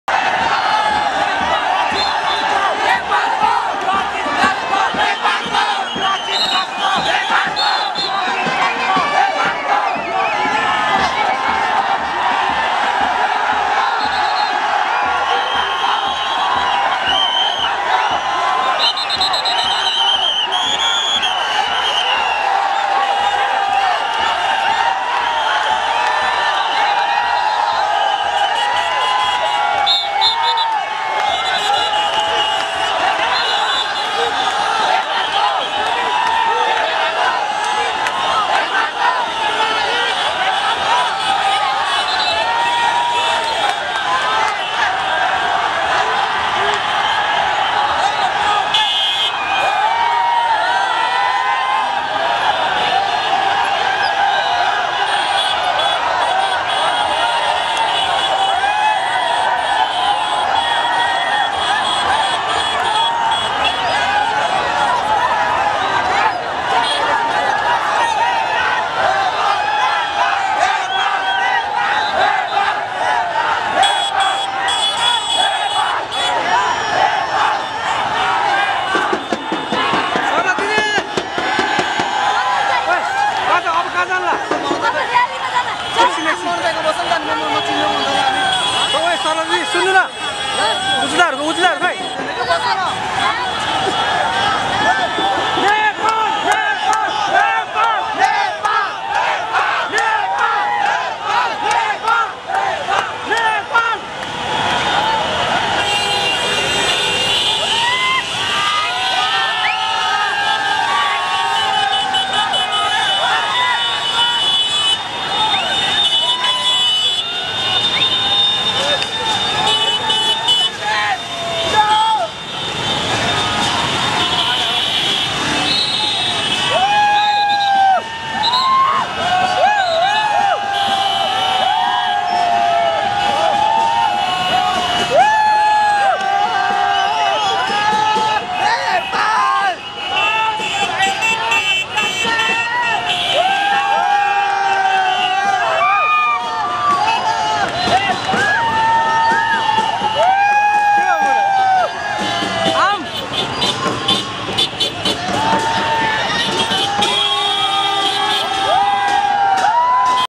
नेपाली फुटबल टिमलाई बिमानस्थलमा भव्य स्वागत: विवेकशील नागरिकहरुको उदाहरण
नेपाली फुटबल टिमलाई बिमानस्थलमा हामी सबै नेपालीहरु मिली यसरी स्वागत गर्यौँ !
देशको शान बढाउने सबै साग खेलाडीहरुलाई हाम्रो सलाम !

नेपालको लागि लड्ने सहि नेतृत्वलाई श्रद्धा गर्ने धन्यवाद दिने, सहयोग गर्ने संस्कार सबै तिर बढोस् नेपाली समाजमा! हाम्रो मनकामना यहि हो !

 विवेकशील नेपाली दल